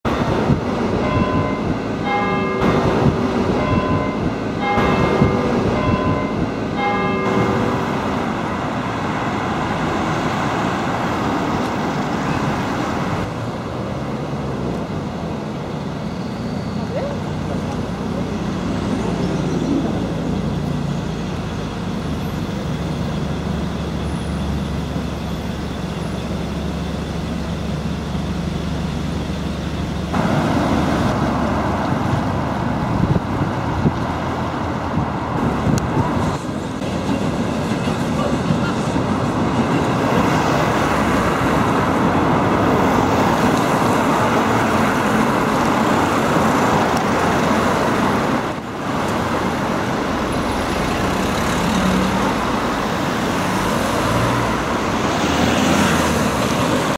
Ian Riley's 'Black Fives' Nos. 44871 & 45407 Passing Newcastle (5Z46) - 9th May 2015
Ian Riley's two 'Black Fives' Nos. 44871 and 45407 pass north on the East Coast Main Line beside the Castle keep at Newcastle. The steam locos were en route from York NRM to Fort William (5Z46) ready for 'The Jacobite' rail tours in Scotland this summer.

A very quickly taken video as I just managed to catch it by seconds so quality and composition not perfect.